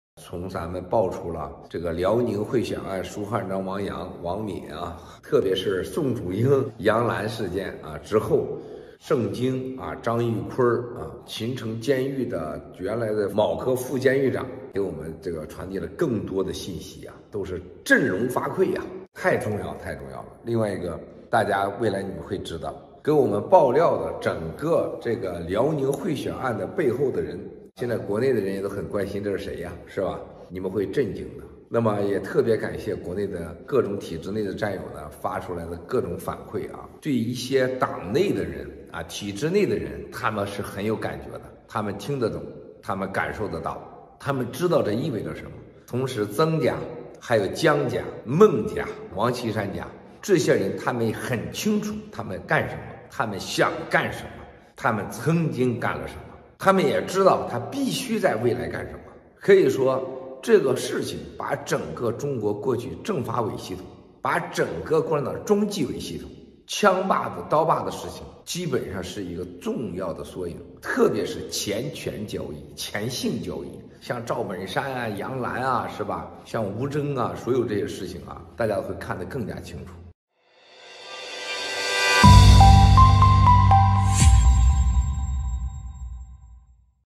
8/26/2022 文贵盖特：体制内的人都听得懂咱们爆的辽宁贿选案以及王珉、宋祖英、杨澜、吴征和赵本山等人的料！
💥8/26/2022 Miles Guo's GETTR: People within the CCP establishment understand the significance of our exposure of the election bribery case in Liaoning and the involvement of Wang Min, Song Zuying, Yang Lan, Bruno Wu, and Zhao Benshan etc. Our exposure is just a miniature of how the CCP’s Central Commission of Discipline Inspection and Political and Legal Affairs Commission, dubbed as the “guns and knives of the CCP”, have been functioning in the past decades.

8/26/2022 文贵盖特：体制内的人都听得懂咱们爆的辽宁贿选案以及王珉、宋祖英、杨澜、吴征和赵本山等人的料；这是中共过去几十年所谓的刀把子和枪把子(即中纪委和政法委)的一个重要缩影！